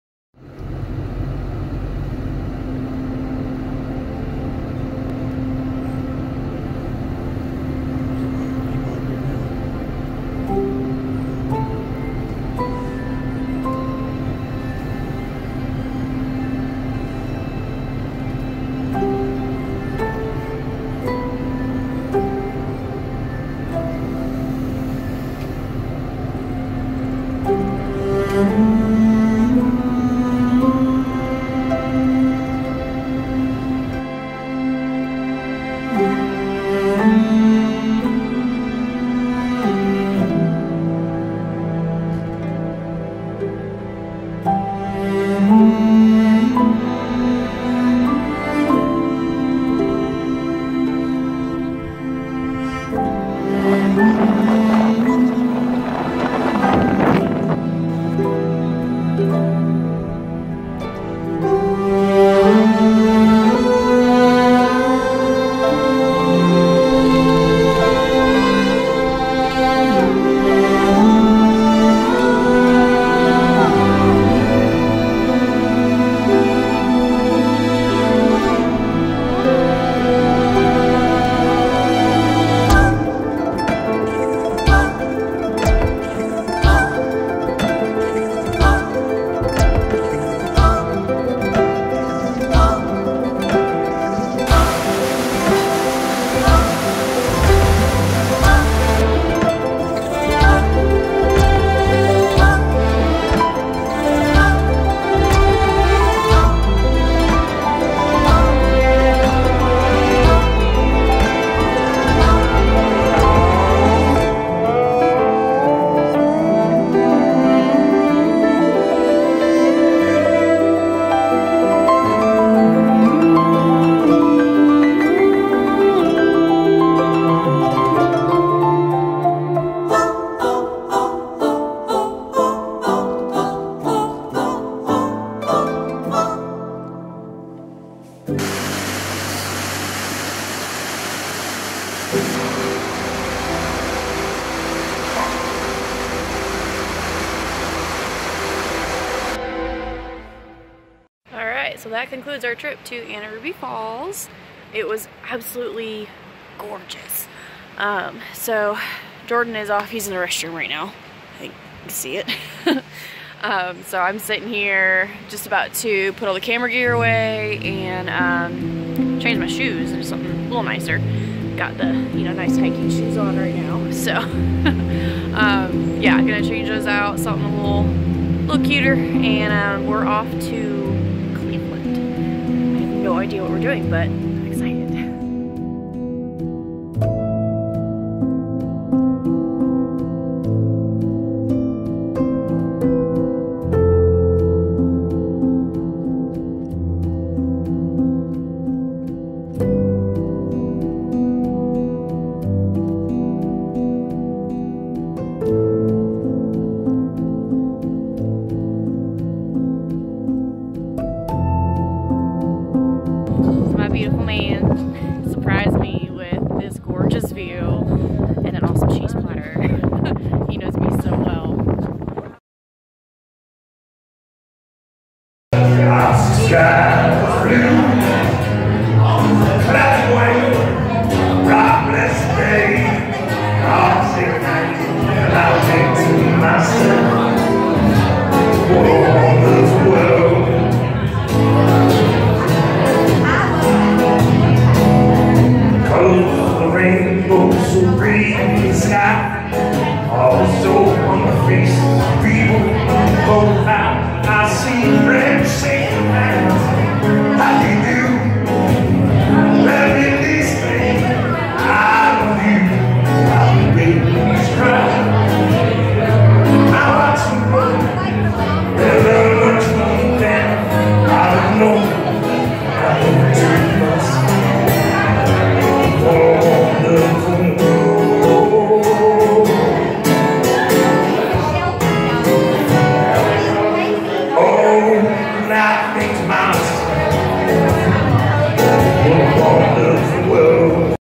Helen Vlog
We went to Helen for Morgan's birthday last year and never got to work on the video. Thanks to the Corona Virus going on now, we got some time to pump out some personal projects like this! Please excuse some of the cellphone footage from a while ago for the slowmo at the beginning.